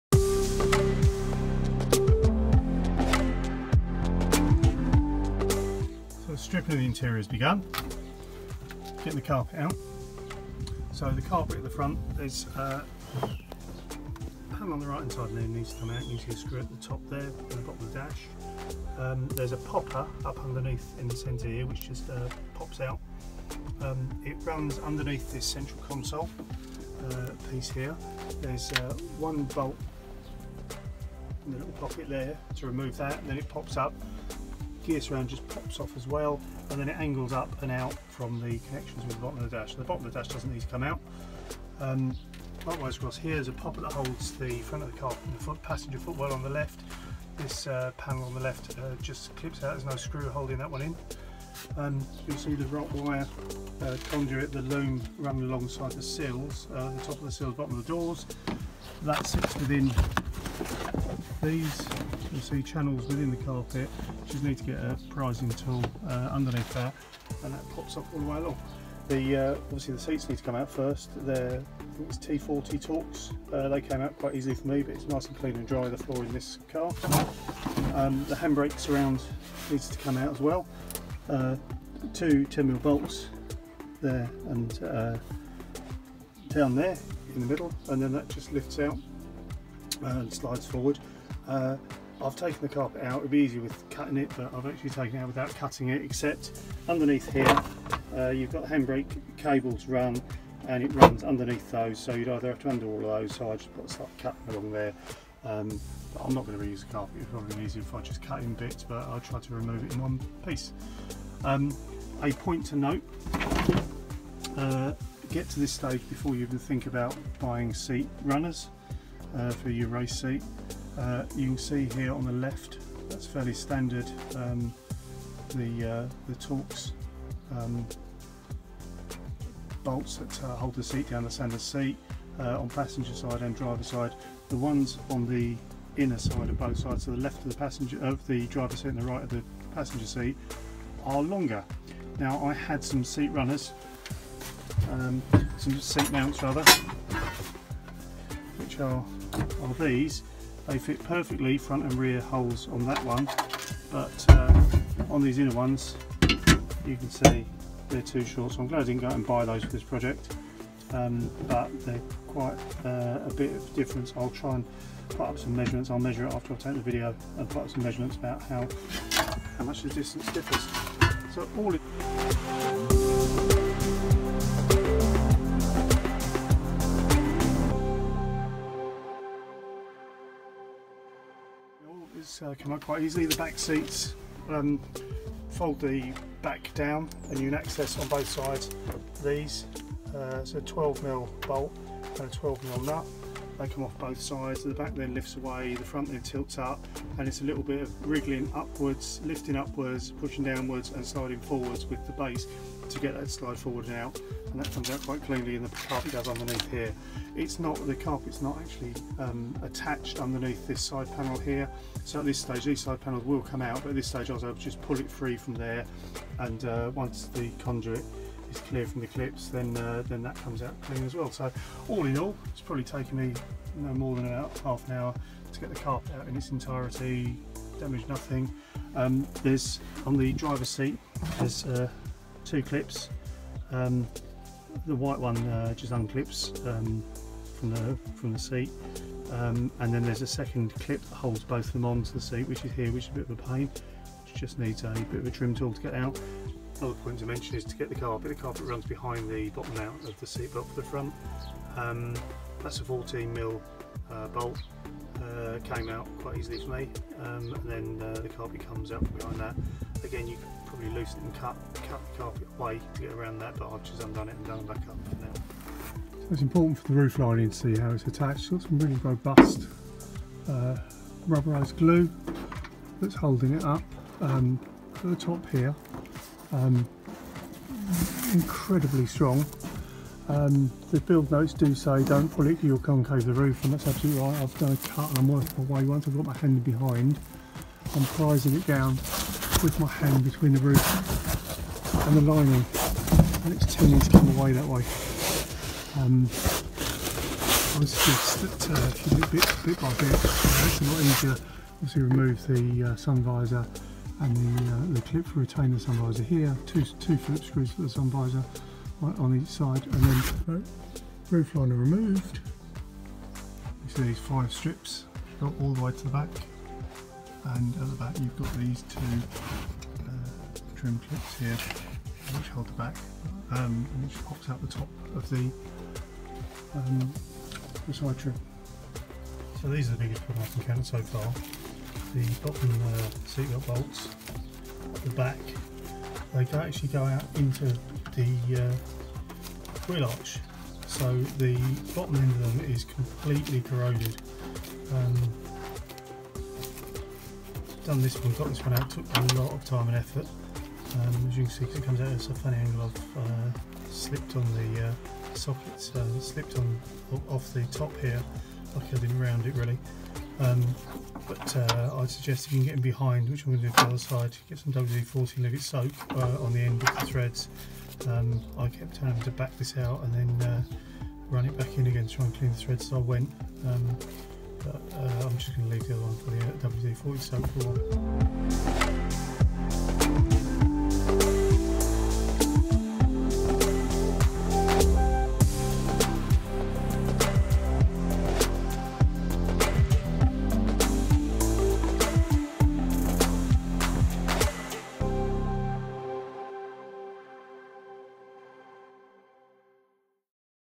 Citroen C1 Race Car Build Part 2 - Stripping Out the Interior
DIY Stripping out the interior and starting to prepare to build a race spec C1 for the C1 Racing Endurance Series